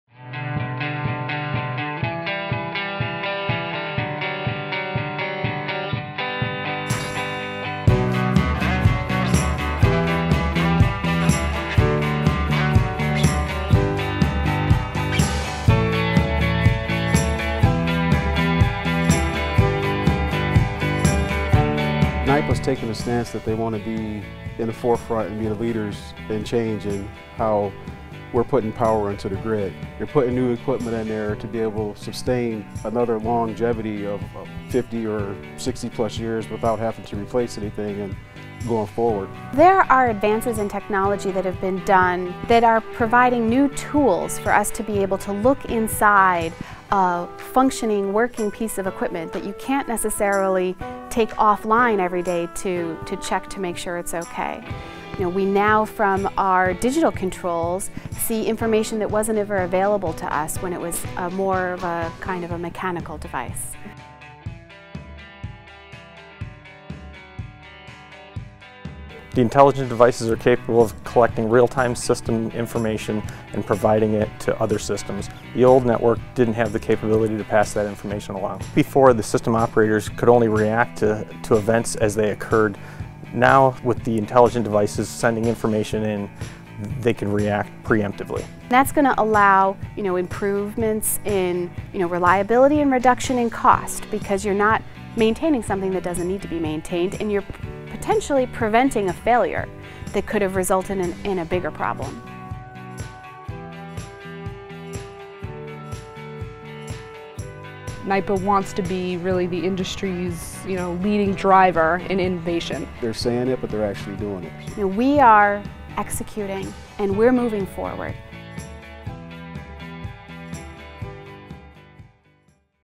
Infrastructure Modernization
More than 80% of New York’s high voltage transmission is over 30 years old, and our generation facilities are over 50. Upgrading NYPA’s generation & transmission assets will increase physical and cyber resiliency.